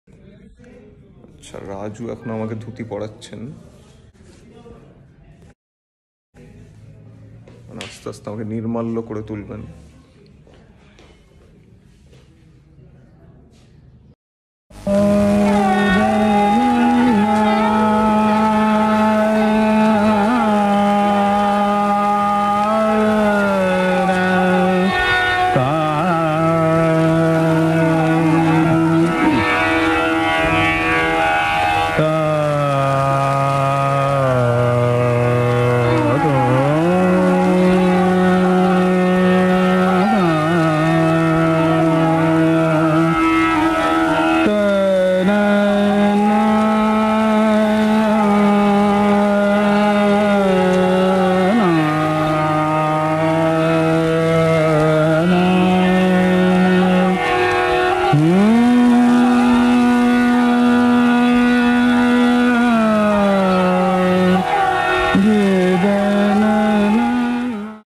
Souman Bose as Nirmalya Acharya in Abhijaan
Nirmalyo Acharya & Soumitra Chatterjee were inseparable friends. Their friendship resulted in an array of memorable & pertinent creative collaborations. Together, they co-founded the magazine ‘Ekkhon’ in 1961. Satyajit Ray had named the magazine & had also designed the inaugural cover pages of the magazine & continued to do so for the subsequent publications. Most of Ray’s film scripts were published in this magazine, amongst other credible content. Souman Bose essays the character of Nirmalya Acharya in Obhijaan, directed by Parambrata Chattopadhyay. Here are a few behind the scene snippets from the film’s shoot.